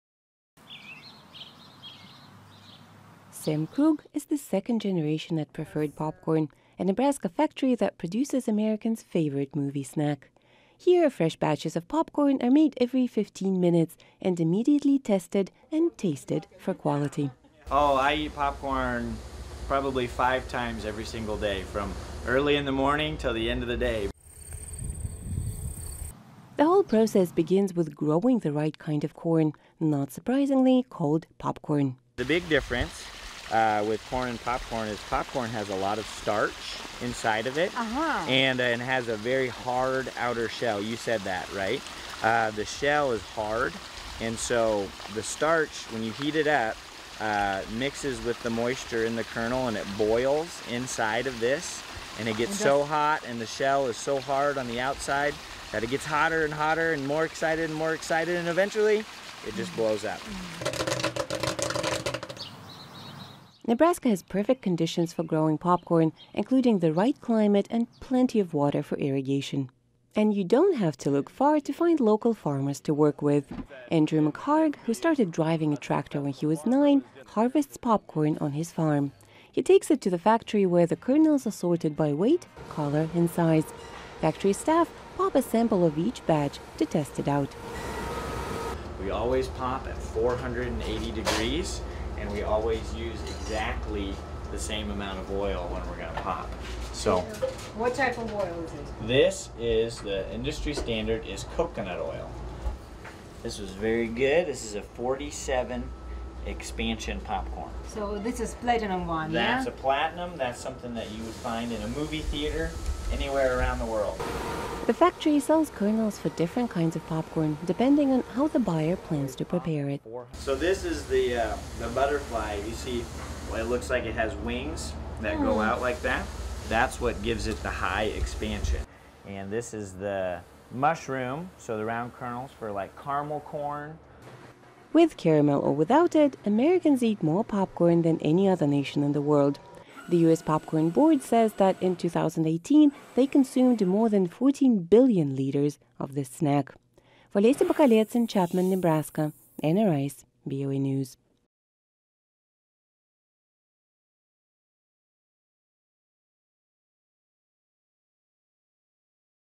Making America's Favorite Movie Snack
No U.S. state produces more popcorn than Nebraska. The factory called Preferred Popcorn smells like a giant movie theatre and is a major producer in America. Lesia Bakalets traveled to Nebraska to find out how it's all done.